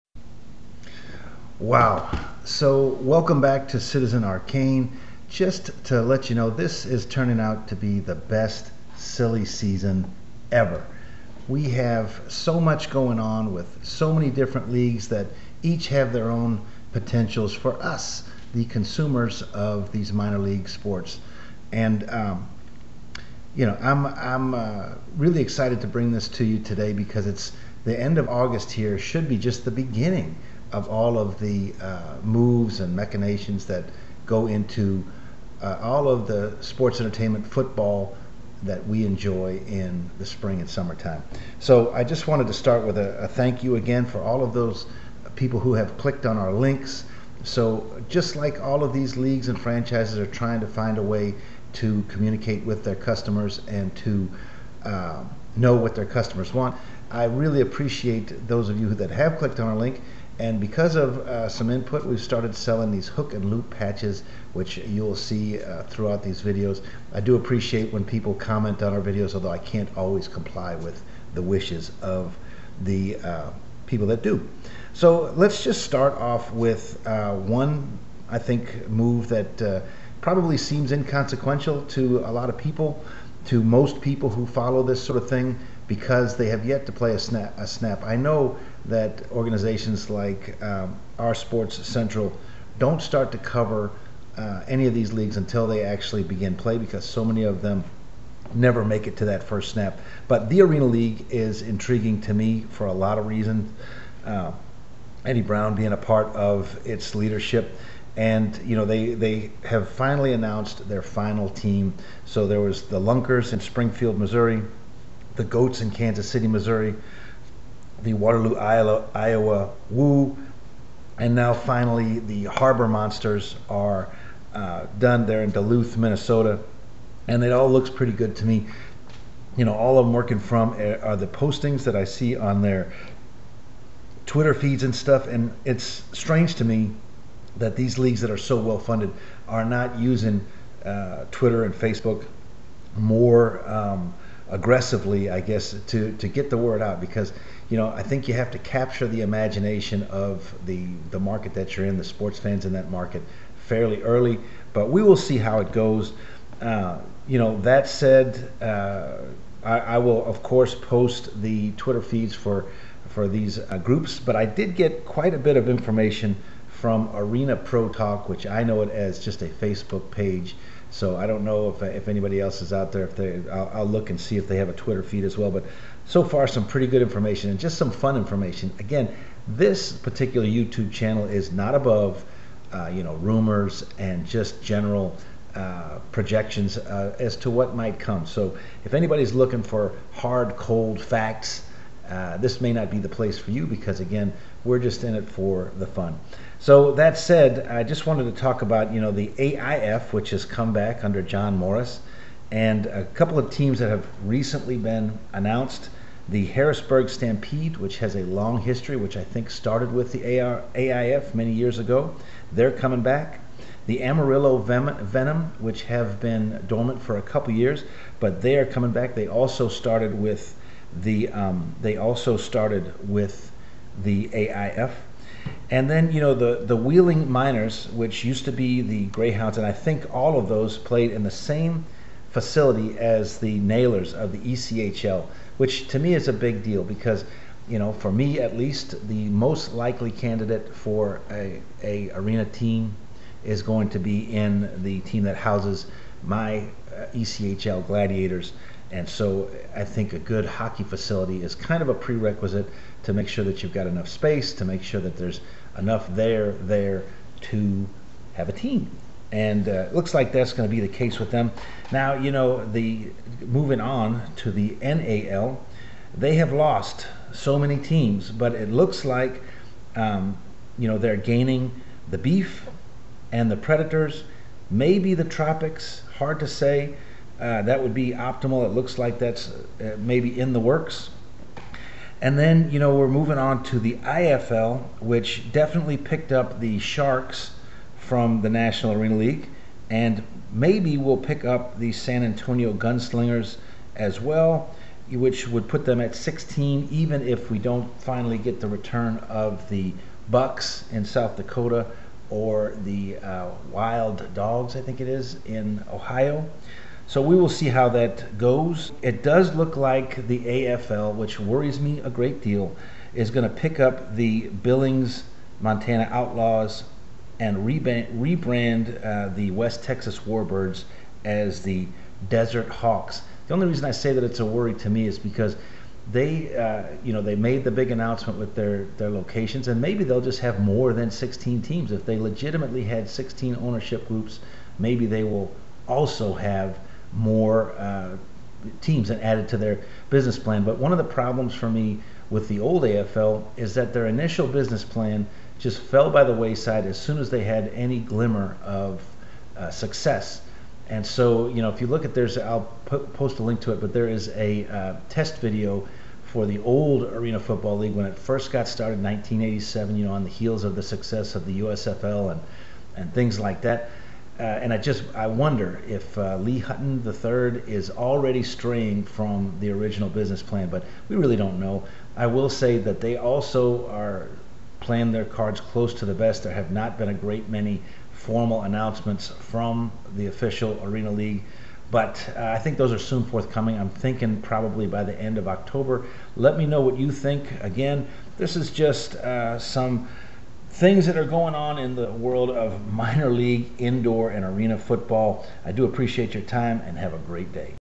August Indoor / Arena Football Update Part III
More updates about the world of Arena and Indoor football teams.  Silly season is in full swing and this is what is going on.